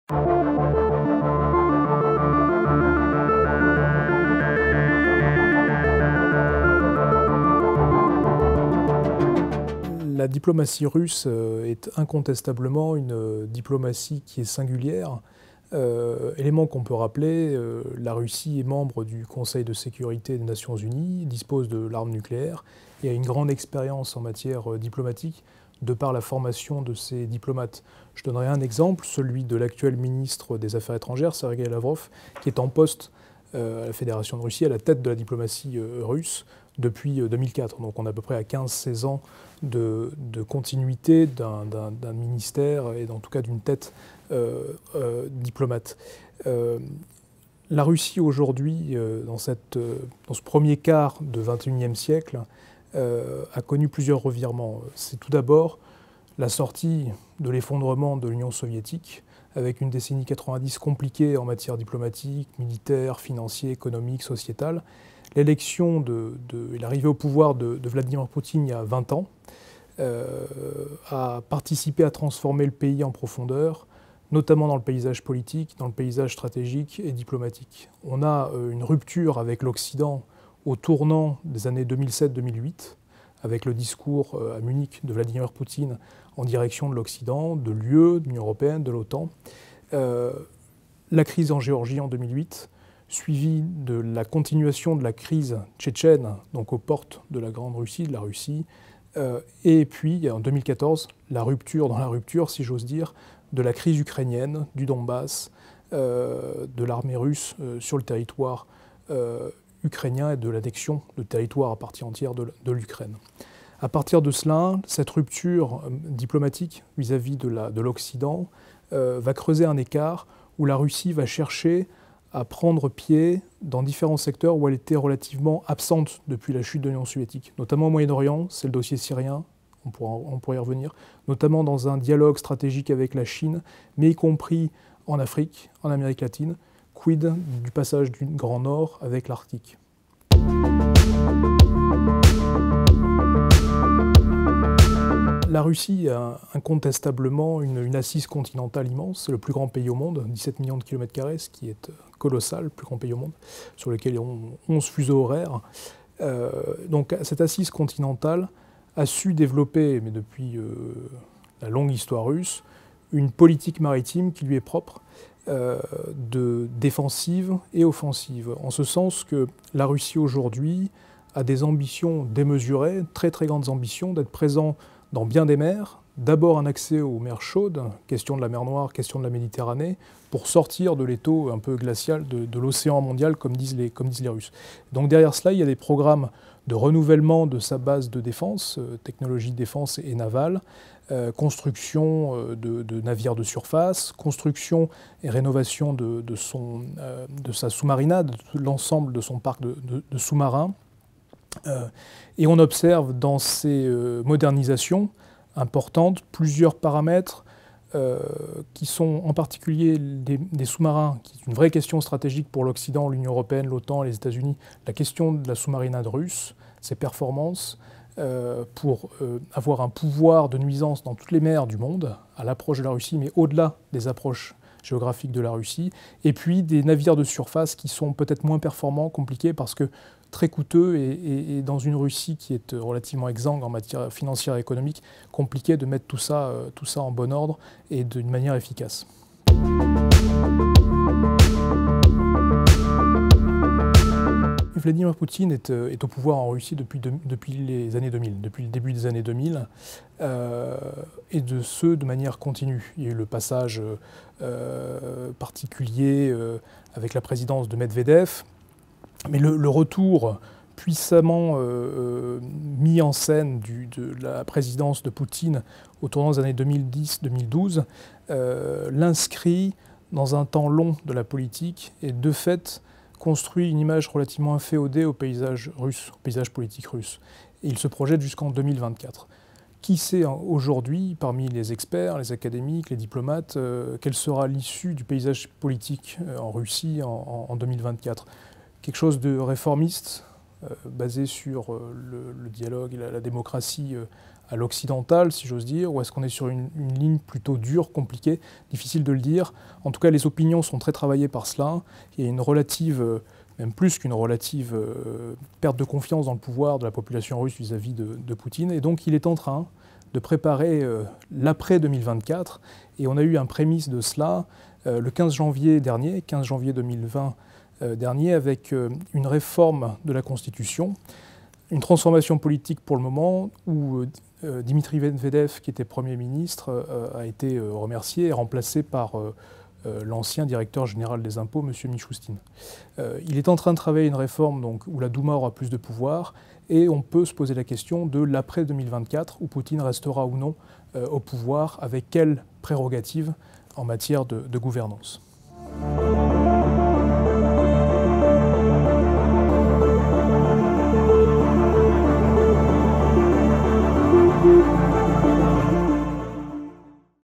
3 questions pour Emmanuel Véron - Russie : nouvelle diplomatie et conséquences en Méditerranée
Emmanuel Véron, docteur en géographie et spécialiste de la Chine contemporaine et de géopolitique. Il est anciennement responsable de la géographie et de la géopolitique de la Chine à l’Inalco/Langues’O, et a enseigné la géopolitique et les relations internationales à l’École navale, dont il est associé. Il est délégué général du Fonds de dotation Brousse dell’Aquila et a été directeur scientifique de la Société de Géographie.

«La Russie : sa nouvelle diplomatie et ses conséquences en Méditerranée» :
La Russie est un acteur important des relations internationales de ce premier quart du XXIe siècle. Si son économie (PIB de l'Espagne), essentiellement issue de l'industrie de l'armement et de la rente des hydrocarbures et autres ressources naturelles, est toute relative au regard de son implication dans de grands dossiers diplomatiques, sa politique internationale prend une place toute particulière dans l'espace méditerranéen : diplomatie, militaire et question de sécurité. La présence navale russe est un facteur très important, singulier et éloquent. Nous présenterons les enjeux et les nouveaux équilibres de cette présence.